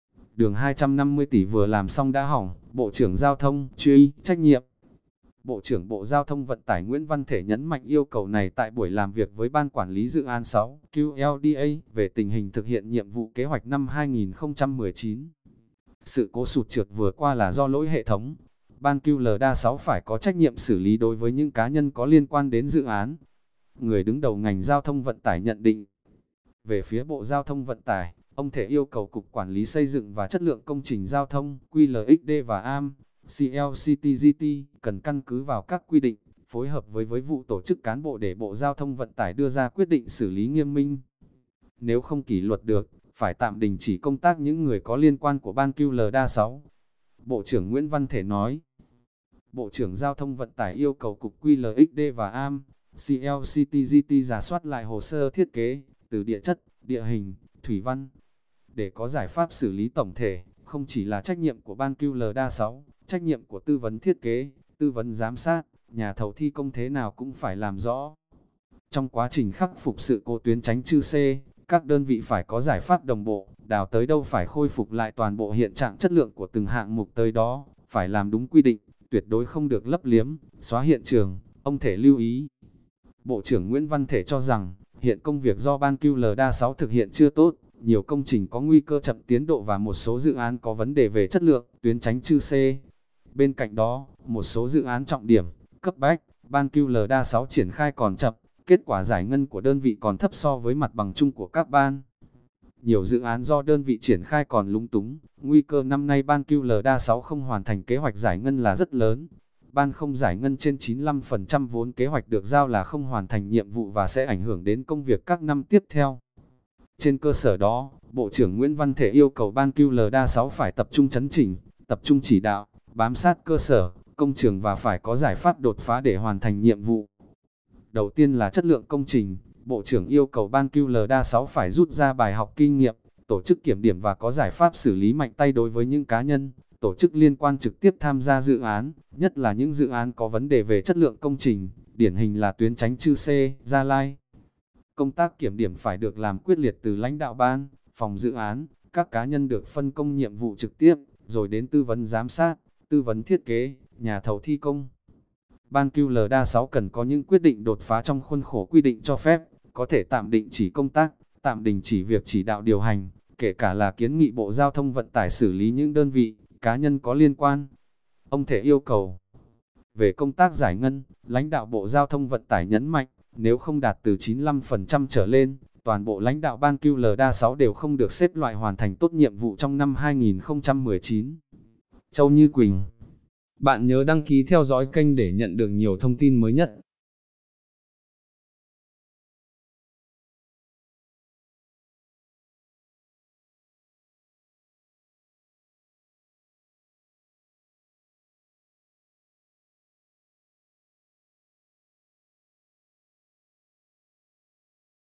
Đường 250 tỷ vừa làm xong đã hỏng: Bộ trưởng Giao thông "truy" trách nhiệm
(Dân trí) - Về sự cố đường Hồ Chí Minh đoạn qua Chư Sê vừa làm xong đã nứt toác, Bộ trưởng Giao thông vận tải (GTVT) hôm nay, 10/9, yêu cầu tạm đình chỉ công tác những người có liên quan của Ban Quản lý dự án 6; làm rõ trách nhiệm của tư vấn thiết kế, tư vấn giám sát, nhà thầu thi công.